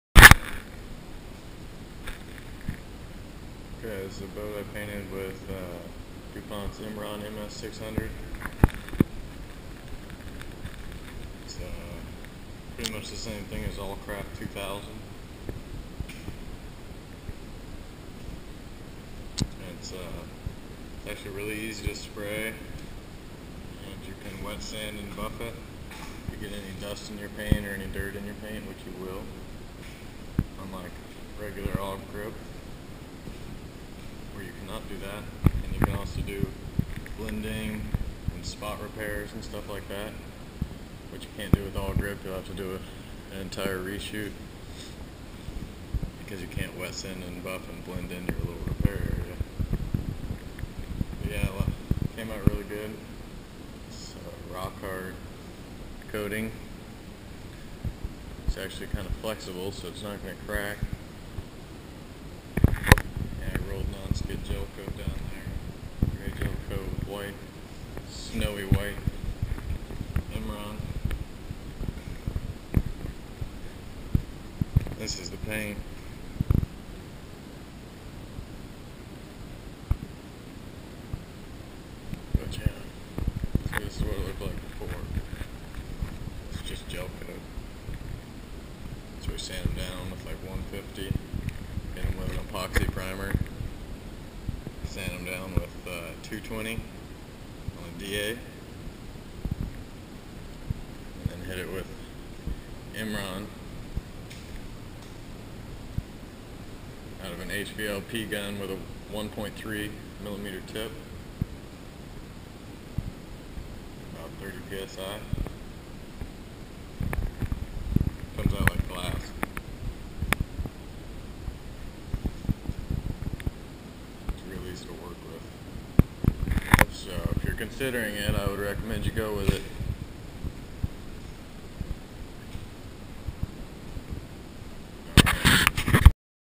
Dupont Imron marine paint finish-MS600
A quick overview of my Dupont Imron MS600 marine paint job. shot through a 1.3mm HVLP DeVilbiss gun around 25-30 psi.  A very similar paint to Awlcraft 2000.